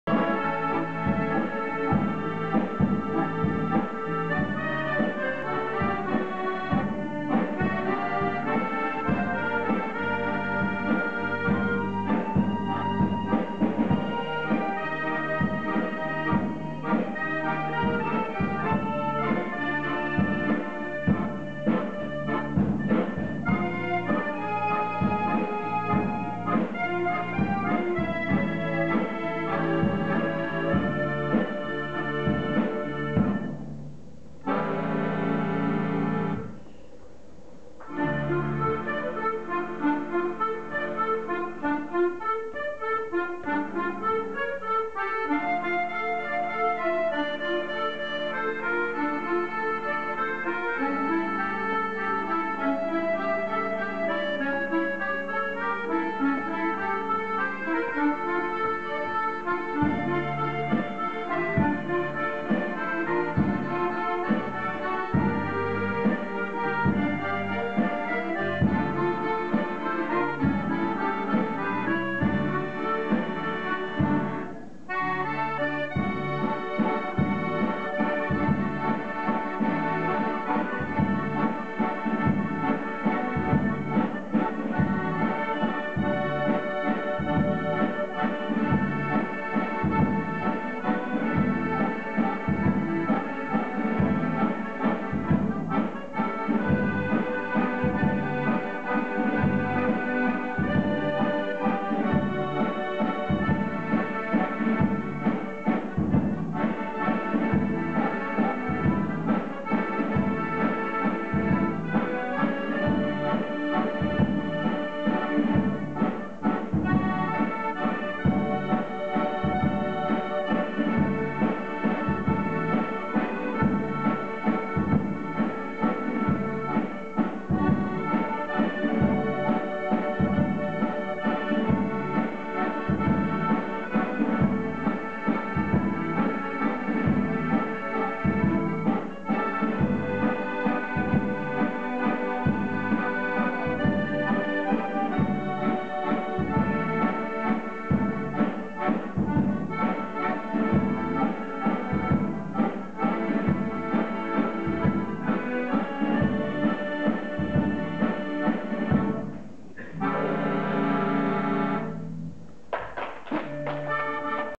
Accord des Coeurs au concert Téléthon / Sidaction de l'école de Musique des Pieux sam. 26/11/11
Orchestre d'accordéons Accord des Coeurs dirigé par Daniel Delbaere lors du concert au profit du Téléthon & du Sidaction, samedi 26 novembre 2011, à l'auditorium de l'école de Musique des Pieux.